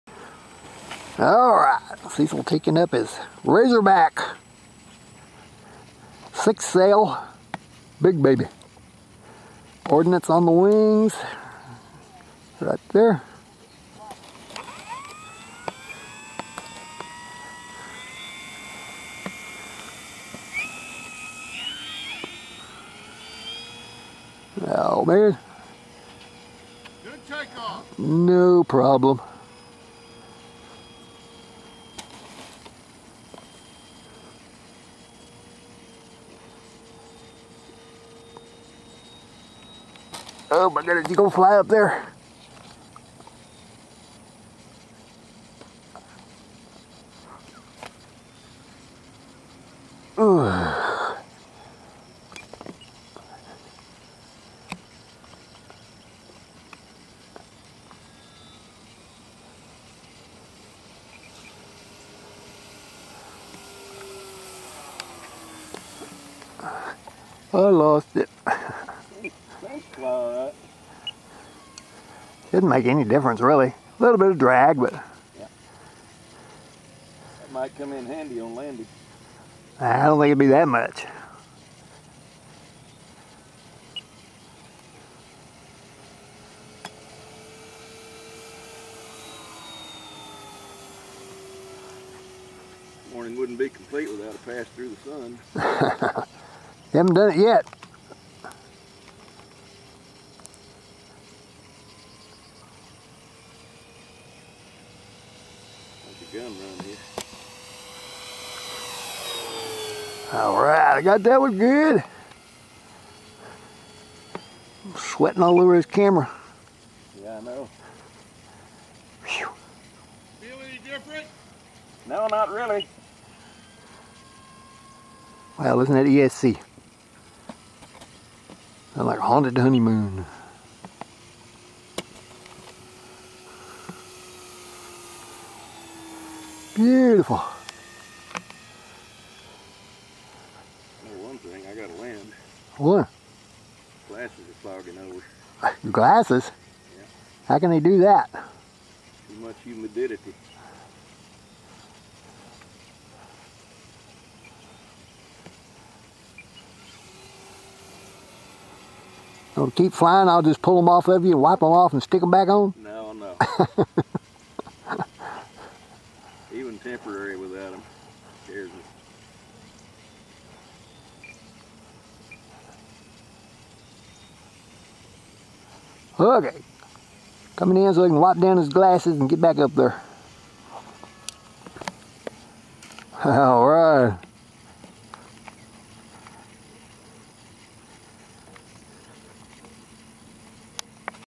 Dawn Patrol 284d
8/24/19. FMS P-47 Razorback Bonnie. Capt. Cecil takes up his new Razorback with armament. Great job of flying, great plane!